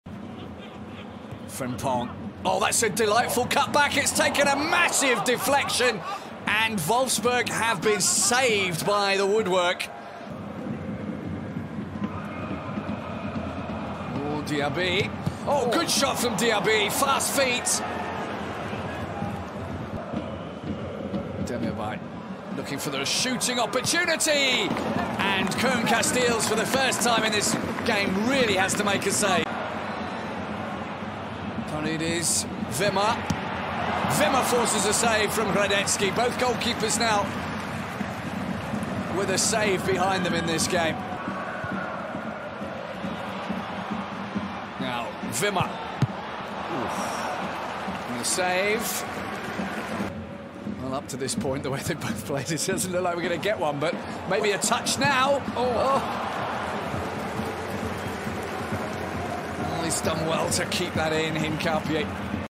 Winning Streak Stopped! | Wolfsburg - Leverkusen 0-0 | Highlights | MD 28 – Bundesliga 2022/23
WOBB04 | Highlights from Matchday 28!

Watch the Bundesliga highlights of VfL Wolfsburg vs. Bayer 04 Leverkusen from Matchday 28 of the 2022/23 season!

Goals: no goals